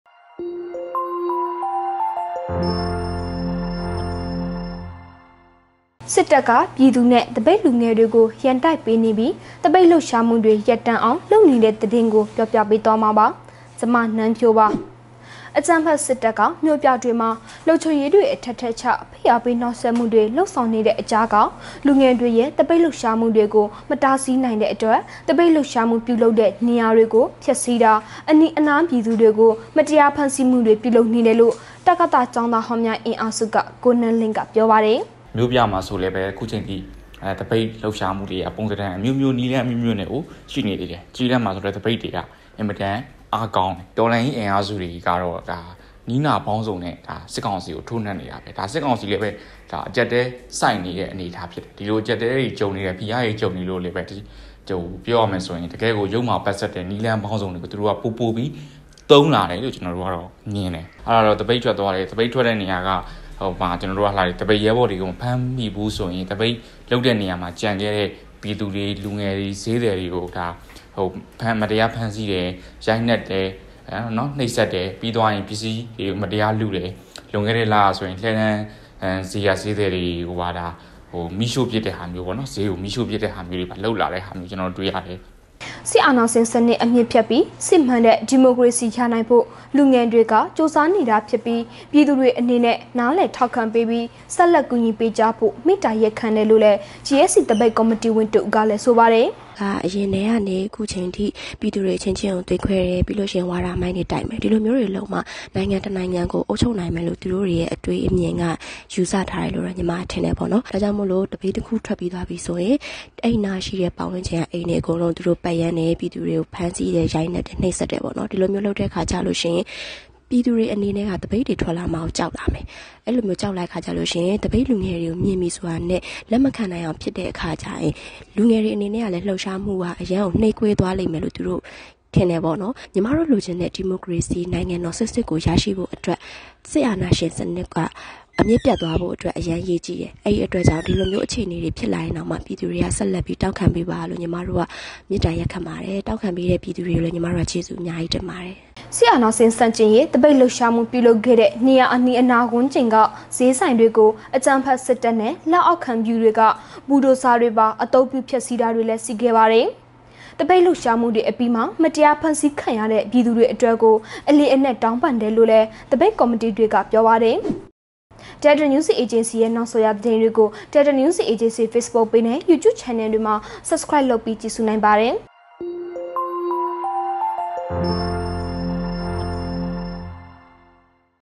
စစ်တပ်ကပြည်သူနဲ့သပိတ်လူငယ်တွေကို ရန်တိုက်ပေးနေပြီး သပိတ်လှုပ်ရှားမှုတွေ ရပ်တန့်အောင် လုပ်နေ
အကြမ်းဖက်စစ်တပ်က မြို့ပြတွေမှာ လုံခြုံရေးတွေအထပ်ထပ်ချ ဖိအားပေးနှောက်ယှက်မှုတွေ လုပ်ဆောင်နေတဲ့ကြားက လူငယ်တွေရဲ့ သပိတ်လှုပ်ရှားမှုတွေကို မတားစီးနိုင်တဲ့အတွက် သပိတ်လှုပ်ရှားမှု ပြုလုပ်တဲ့နေရာတွေကို ဖျက်ဆီးတာ ၊အနီးနားပြည်သူတွေကို မတရားဖမ်းဆီးမှု တွေ ပြုလုပ်နေတယ်လို့ တကသ ကျောင်းသားဟောင်းများအင်အားစုက ကိုနန်းလင်းက ပြောပါတယ်။

စစ်အာဏာရှင်စနစ်အမြစ်ဖြတ်ပြီး စစ်မှန်တဲ့ဒီမိုကရေစီရနိုင်ဖို့ လူငယ်တွေက ကြိုးစားနေတာဖြစ်ပြီး ပြည်သူတွေအနေနဲ့ နားလည်တောင့်ခံပေးပြီး ဆက်လက်ကူညီပေးကြဖို့ မေတ္တာရပ်ခံတယ်လို့လည်း GSC သပိတ်ကော်မတီဝင်တစ်ဦးကလည်း ဆိုပါတယ်။

 စစ်အာဏာရှင်ဆန့်ကျင်ရေးသပိတ်လှုပ်ရှားမှုပြုလုပ်ခဲ့တဲ့နေရာအနီးနားဝန်းကျင်ကဈေးဆိုင်တွေကို အကြမ်းဖက်စစ်တပ်နဲ့ လက်အောက်ခံပျုတွေက ဘူဒိုဇာတွေပါ အသုံးပြု ဖျက်ဆီးတာတွေရှိခဲ့ပါတယ်။ သပိတ်လှုပ်ရှားမှုတွေအပြီးမှာမတရားဖမ်းဆီးခံခဲ့ရတဲ့ပြည်သူတွေအတွက်ကို အလေးအနက် တောင်းပန်တယ်လို့ သပိတ်ကော်မတီတွေက ပြောပါတယ်။